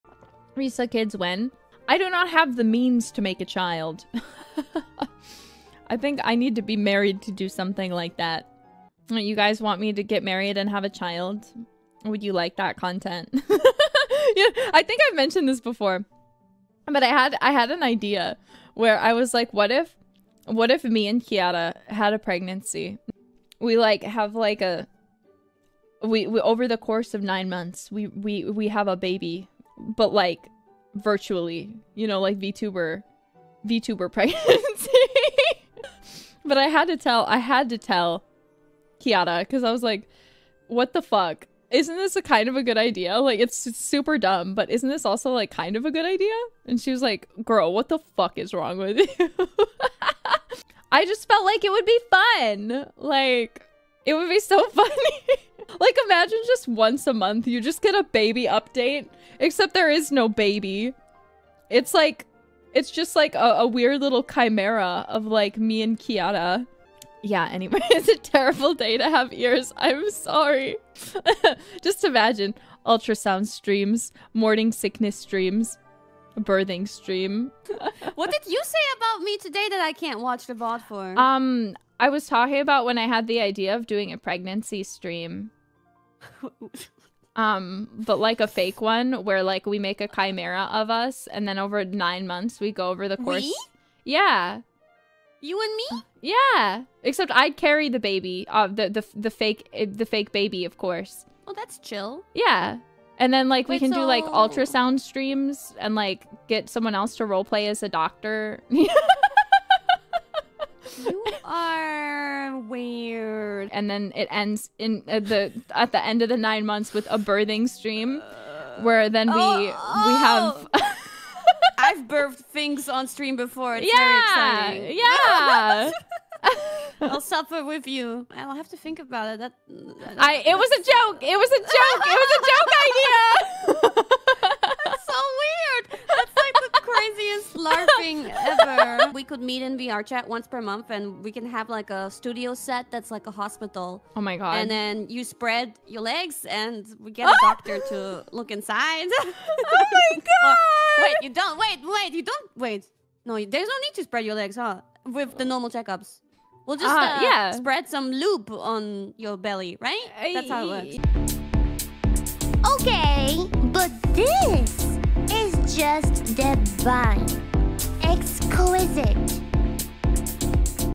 Nerissa Wants To Get Pregnant (With Kiara)
If Nerissa and Kiara do it, a lot of Jailbirds and KFP are going to try to pull off an Oshi No Ko style isekai